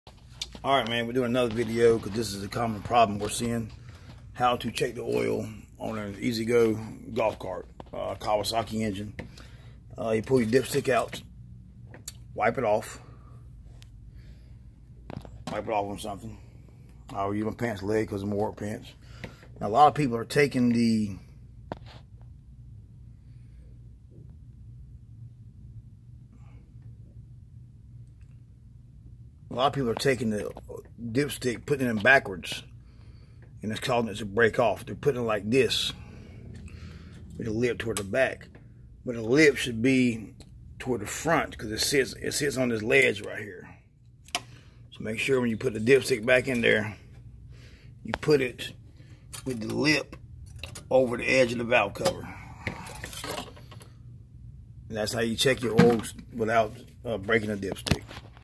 How to check oil on an ezgo golf cart. Kawasaki engine.
How to check the oil easy go golf cart with the Kawasaki engine. A tip be careful because the dipstick only goes in one way if you put it in backwards it will break the dipstick off trying to get it out. Anf you will have to put a magnet inside your drain plug and try to fish it out...